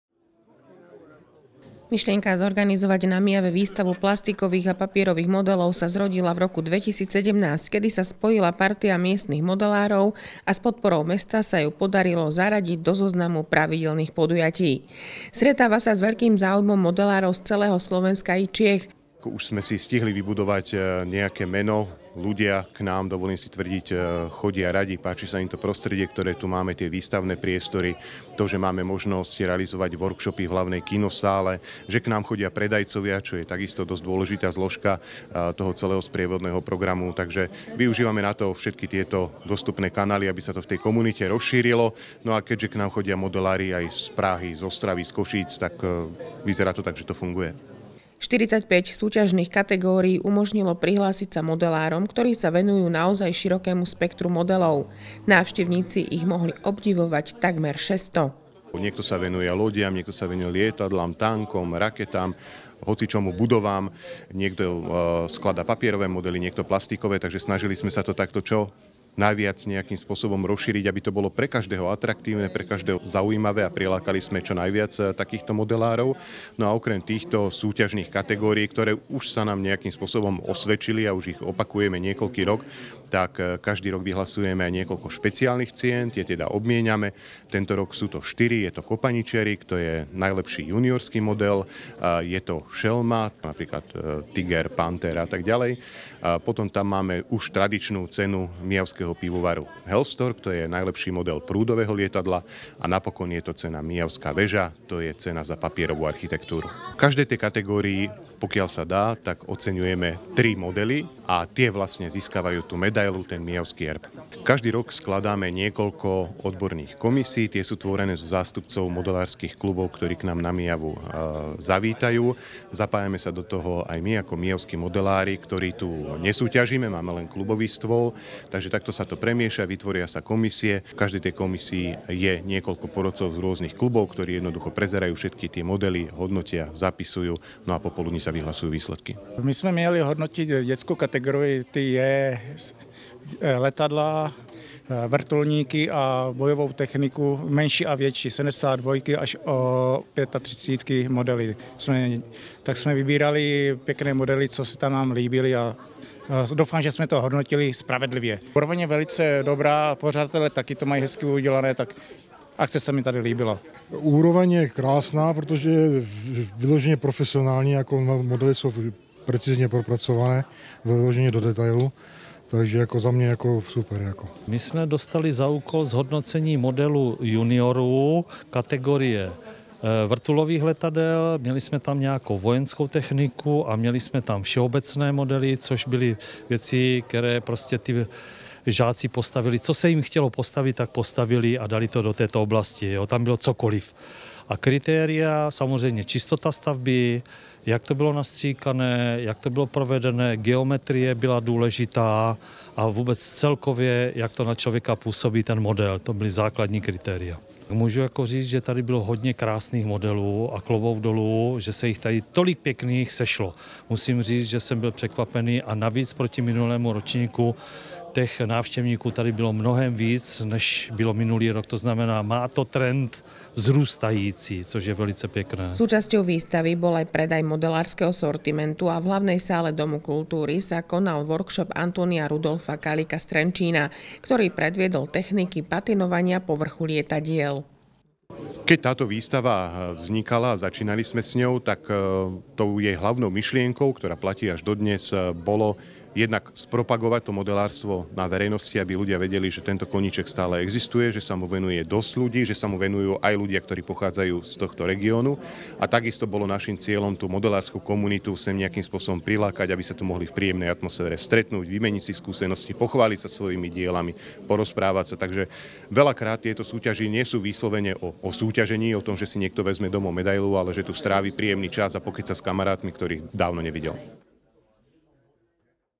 Výstava plastikových a papierových modelov sa konala na Myjave 5 krát
Súťažilo sa v 45 kategóriách